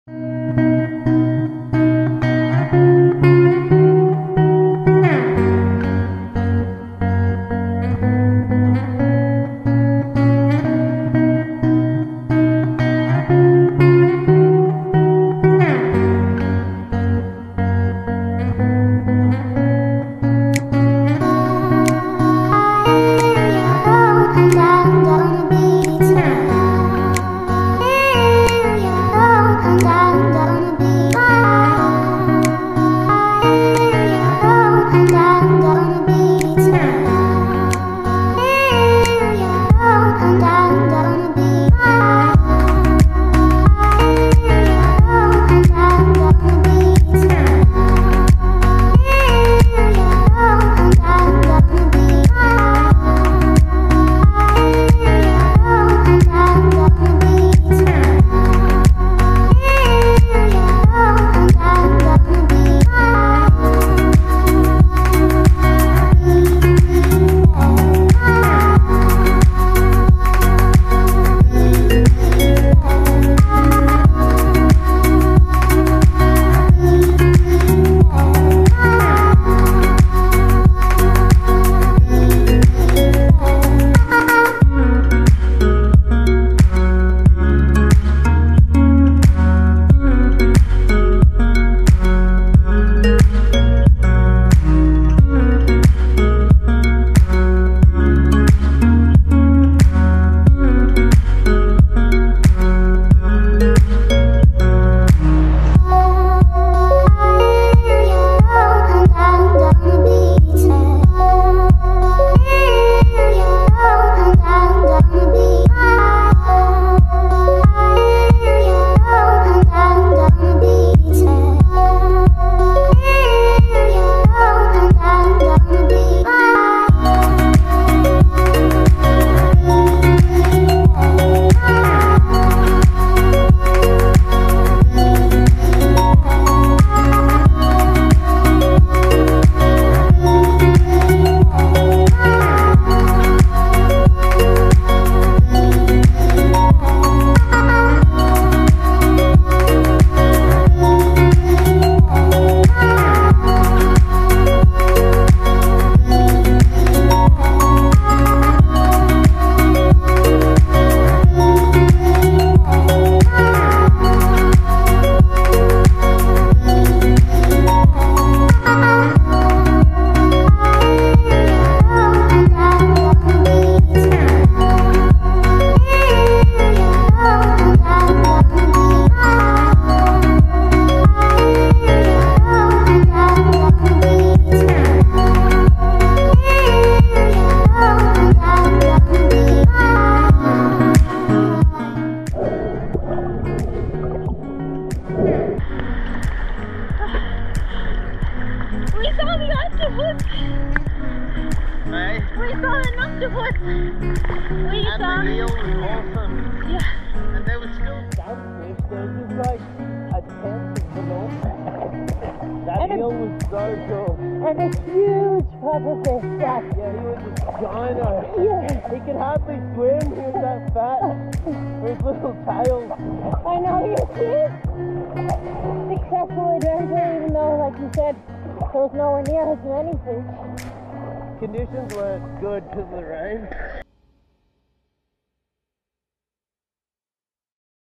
Snorkelling Tallebudgera creek - Octopus wraps his tentacle around my finger, puffer fish & more
December 2023 snorkel at the Tallebudgera creek

A beautiful octopus wraps his tentacle around my finger, there’s puffer fish & eels & more!